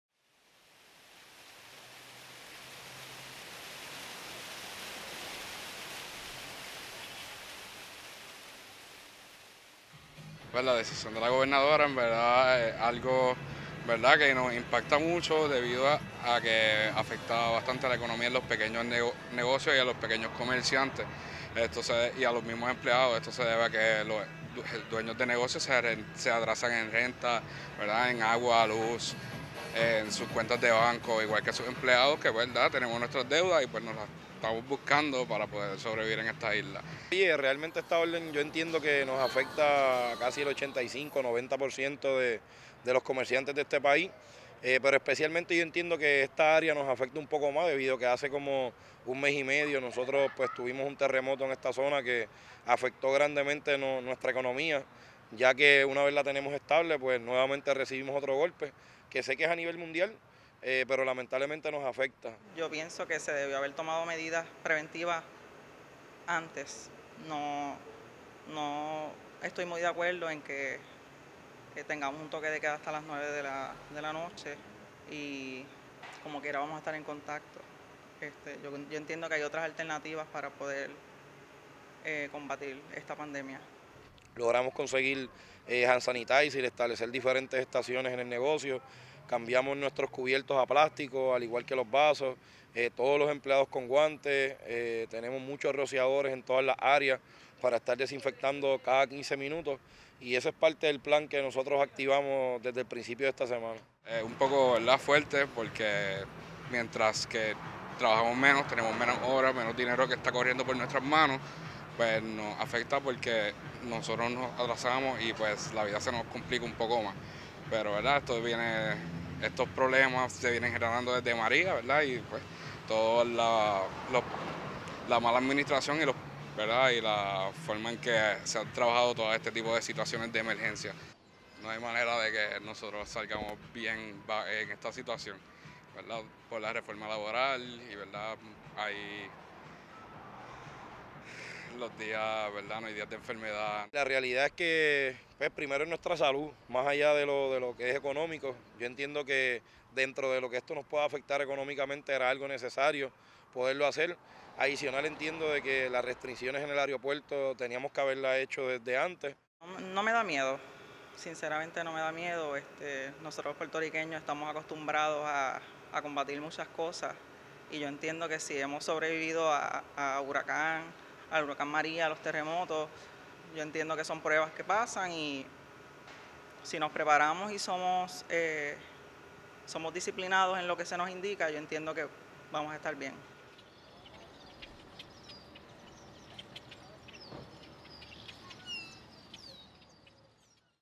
Puerto Ricans brace for effects of coronavirus shutdown
After the government shut down businesses and set a curfew in an effort to contain coronavirus, Puerto Ricans are concerned about what it will mean for an economy still recovering from Hurricane Maria and a series of recent earthquakes.